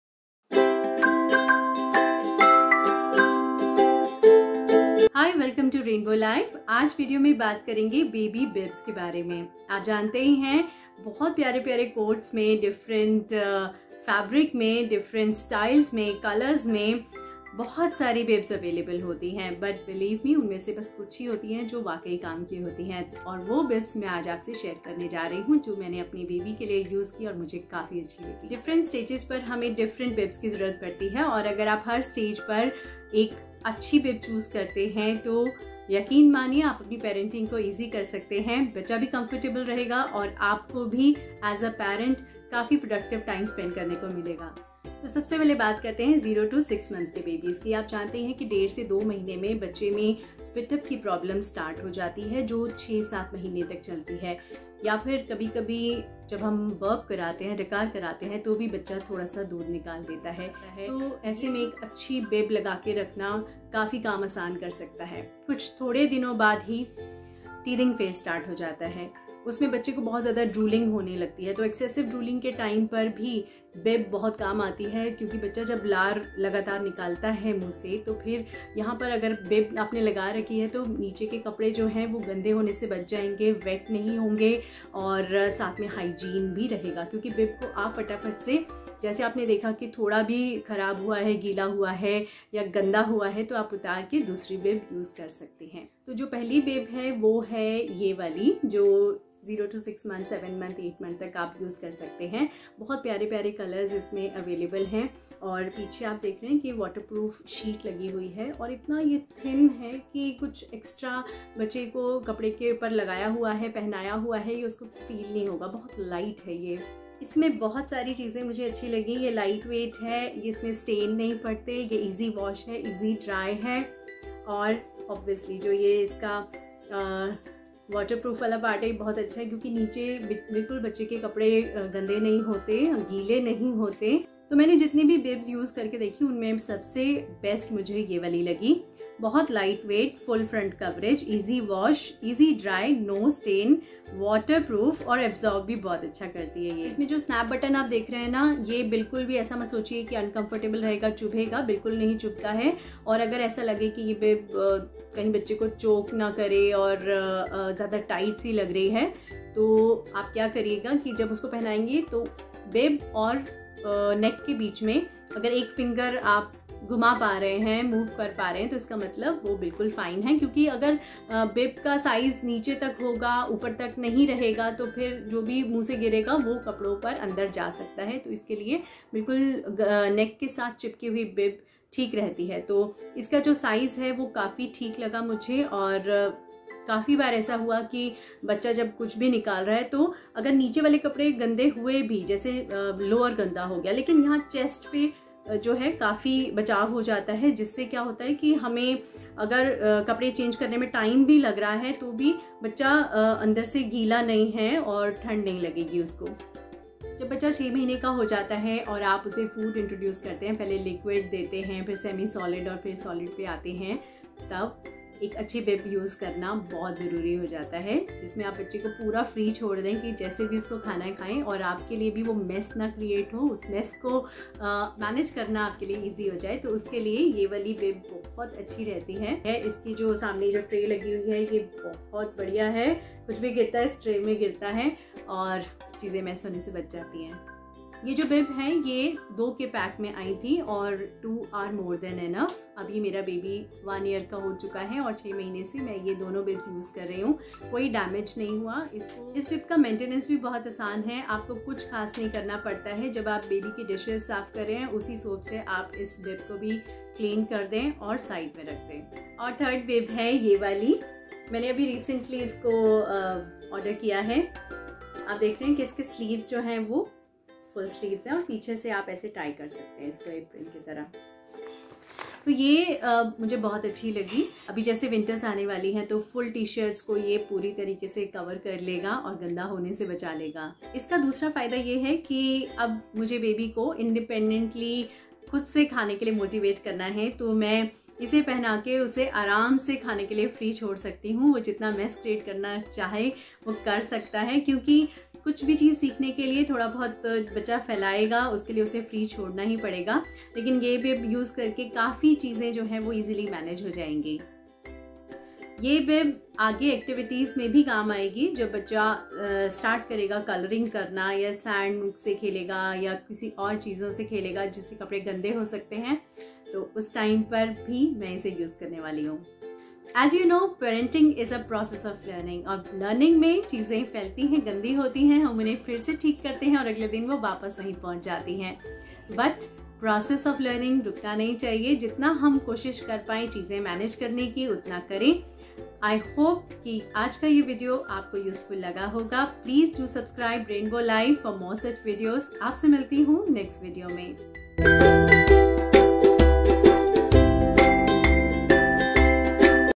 Best Baby Bibs | Feeding Bibs | Waterproof Silicon baby bib | Bibs for babies | Baby Essentials
Cloth and waterproof Bibs

Silicon Bibs:

Smock Bibs :

choose specific Bib for different stage in babies developmental journey.
1. Cloth bibs.
2. Feeding Bibs
3.Smock bibs

1. Cloth bibs are just Simple, machine-washable bibs made from a soft, absorbent material like cotton or muslin.
Potect baby from getting wet at the time of spitup or drooling.

2. Silicone bibs are soft, flexible and waterproof. They can also be wiped clean after mealtime. It has pocket at the bottom to catch food that your little one drops so it doesn’t end up on her lap.

3.Smock bibs are waterproof, wipe-clean bibs with sleeves. They’re a good option for messy eaters since they offer full coverage.

Tip - Clean as soon as possible after use.